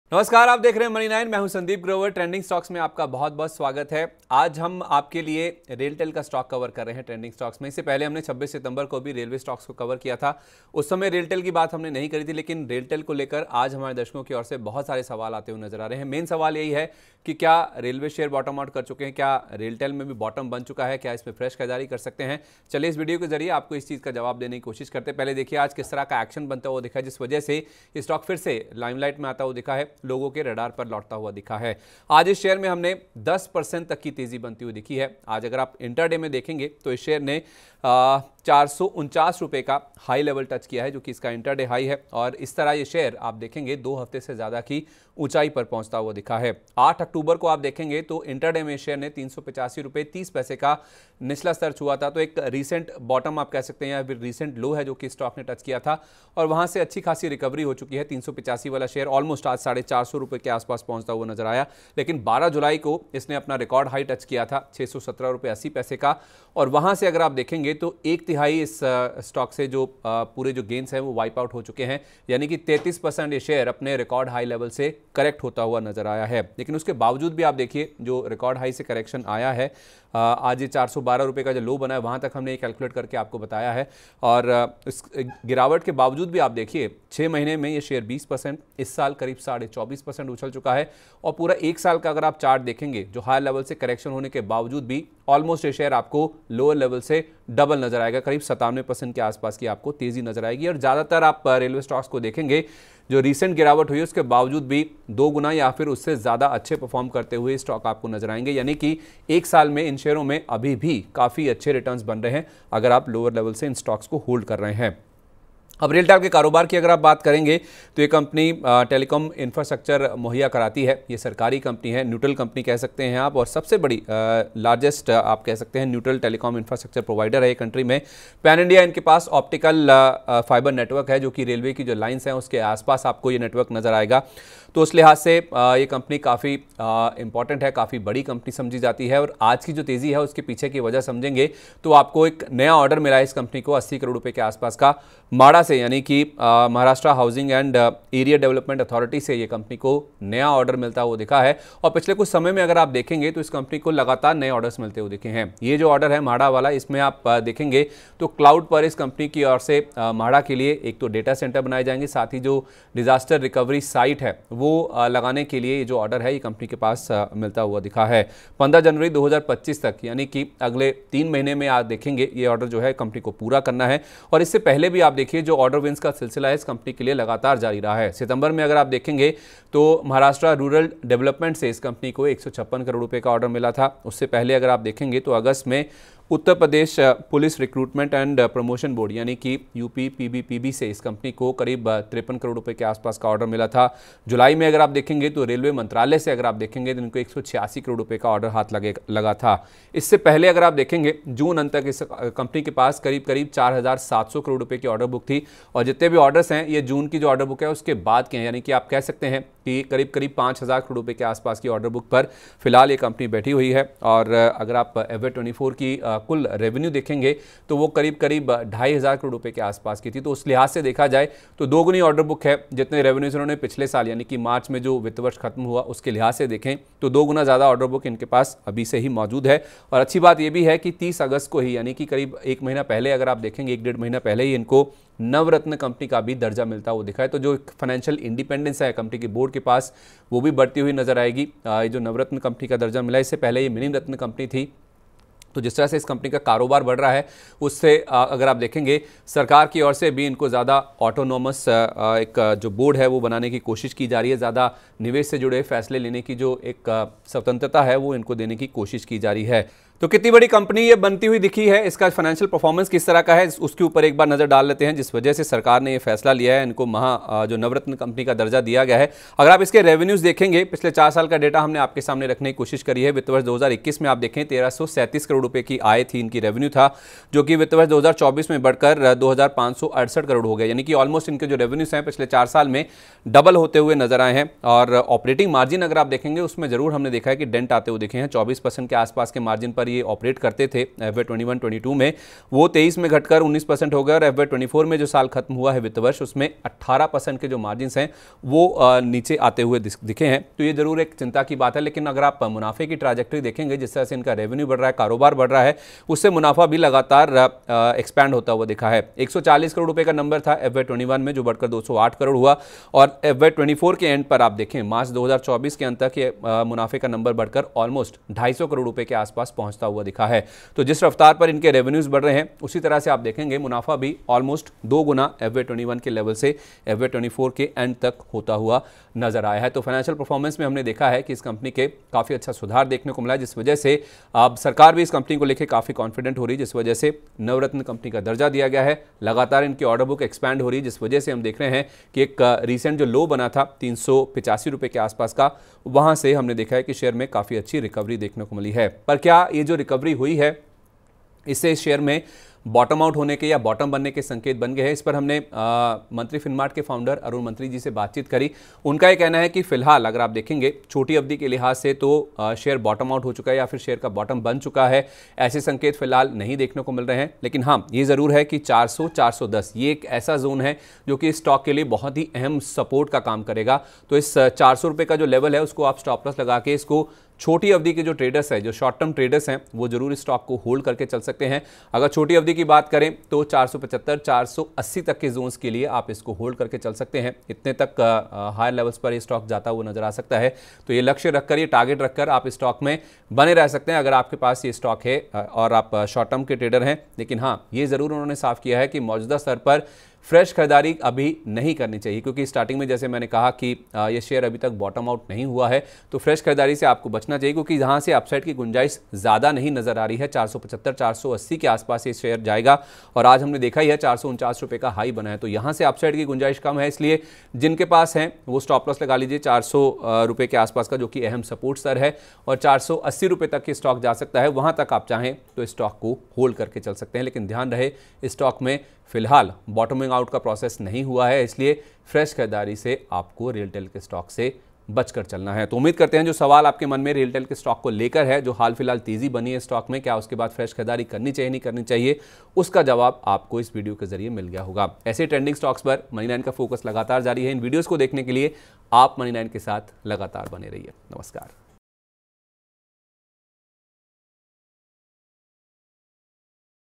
RailTel Corp Share Price | रिकॉर्ड ऊंचाई से 33% गिरने के बाद क्या RailTel Share में बन चुका है बॉटम?
RailTel Share Price : Railtel Share में आज आई तेजी के पीछे क्या है बड़ी वजह? जुलाई में रिकॉर्ड ऊंचाई छूने के बाद Railtel Share में कितनी गिरावट आ चुकी है? रिकॉर्ड ऊंचाई लगाकर फिसले Railtel Share क्या बन चुका है बॉटम? Railtel Share को लेकर  क्या है एक्सपर्ट्स के नए टारगेट? जानने के लिए Video देखें